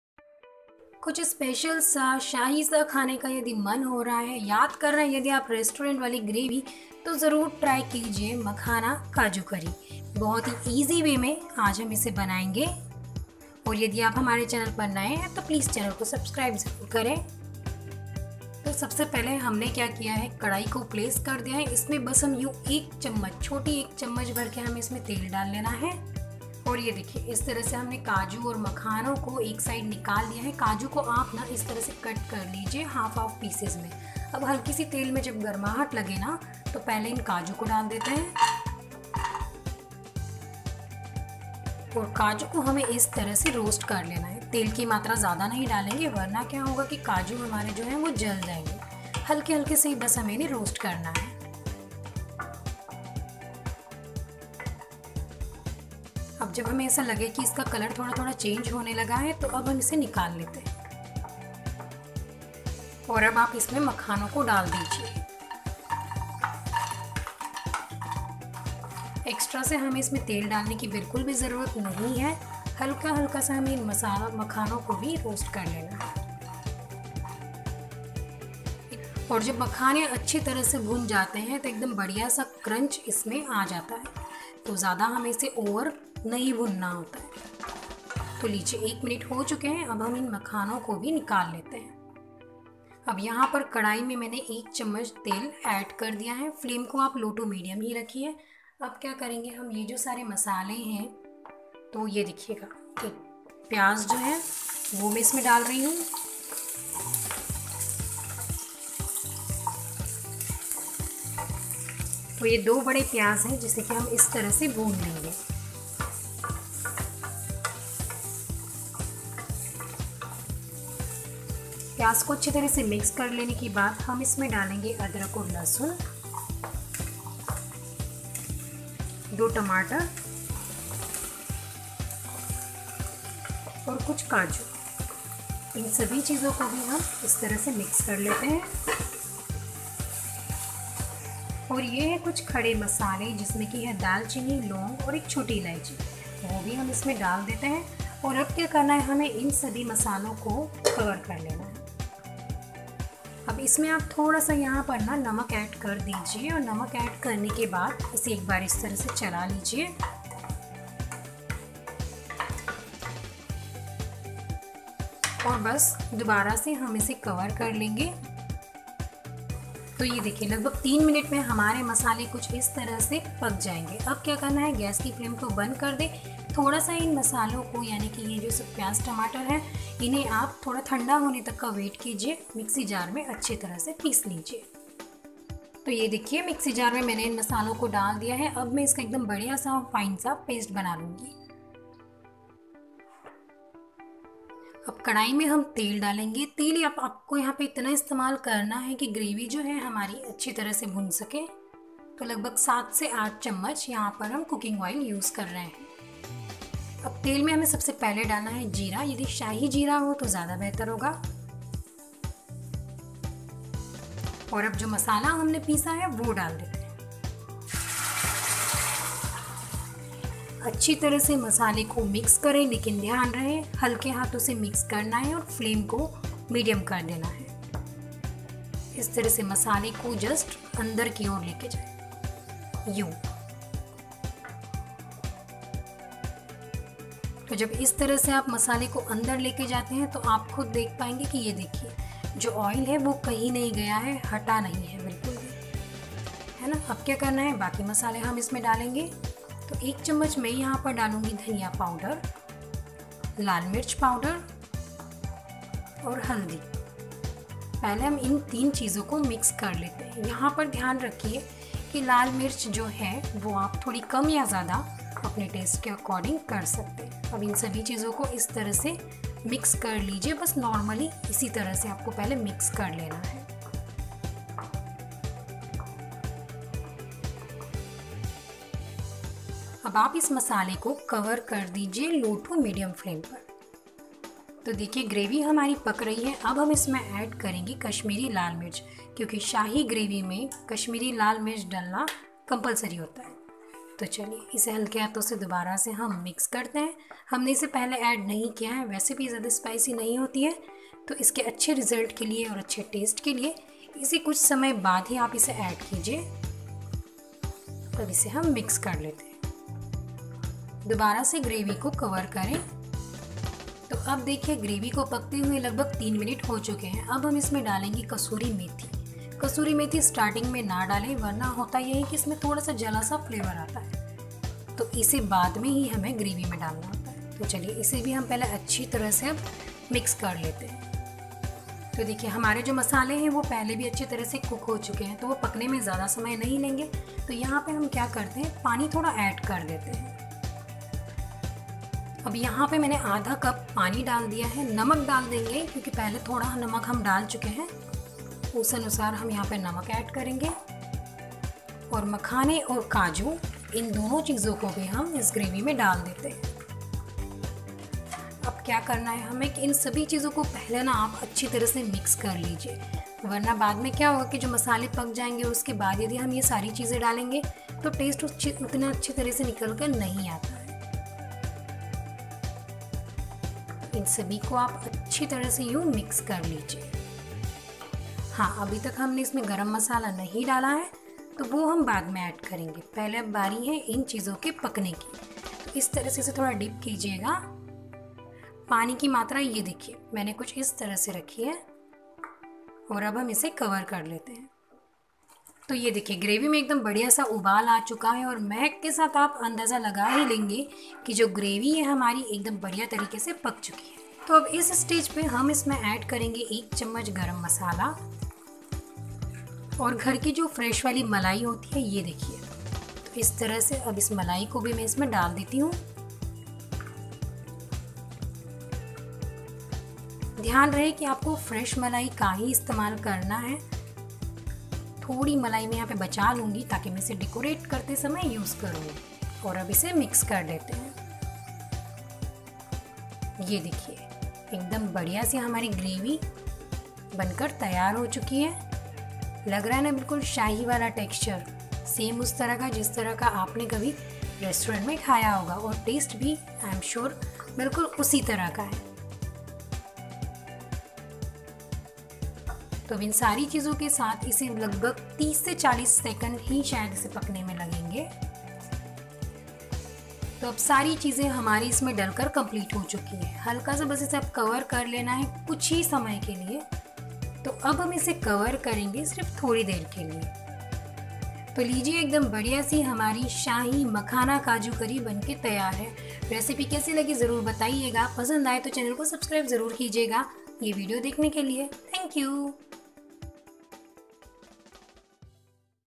Shahi Makhana kaju curry | रेस्टोरेंट वाली method से घर पर बनाये | restaurant style kaju curry
Makhana kaju curry | रेस्टोरेंट वाली method से घर पर बनाये | restaurant style kaju curry at home